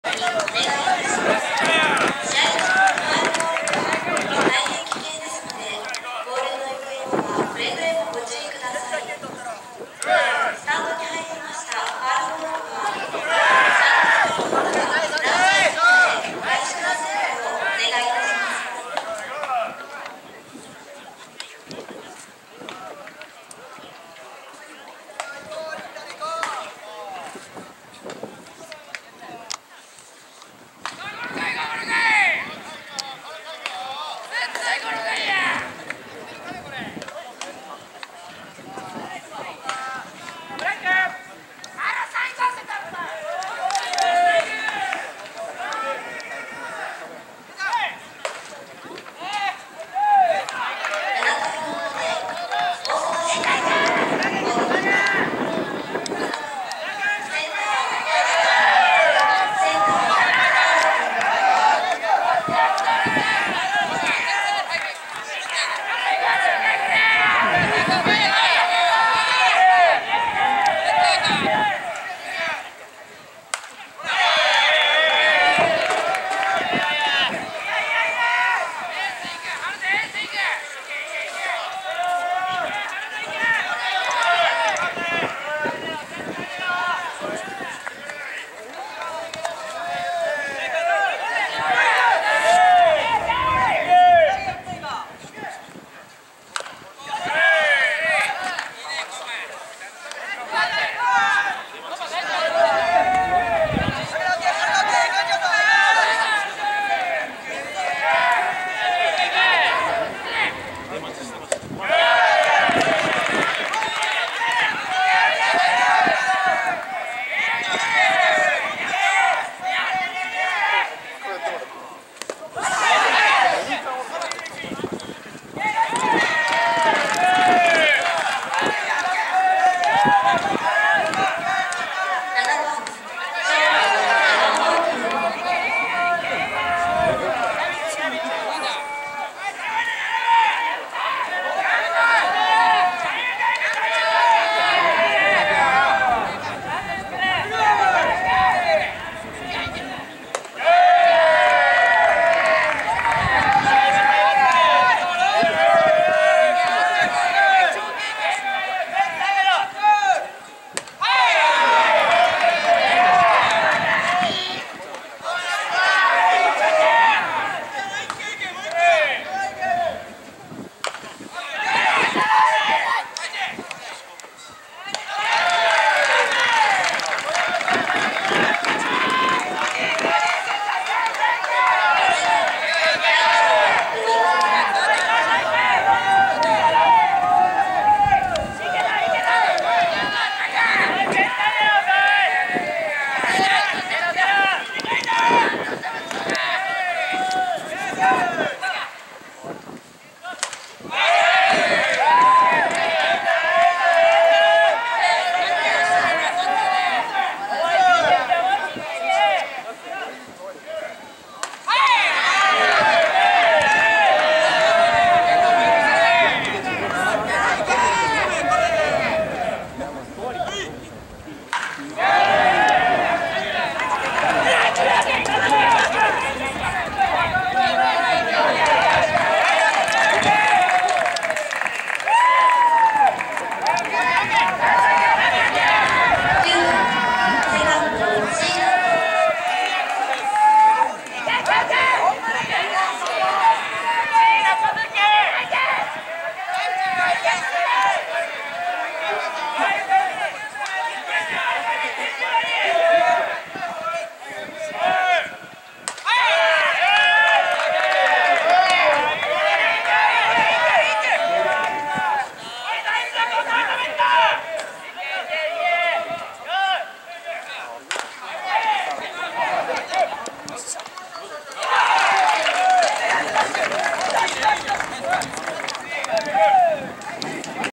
2015/10/12 大阪市立大学vs大阪観光大学 ７回表 大阪市立大学
近畿学生野球二部　秋季リーグ戦
大阪市立大学vs大阪観光大学　２回戦　舞洲ベースボールスタジアム
７回表　大阪市立大学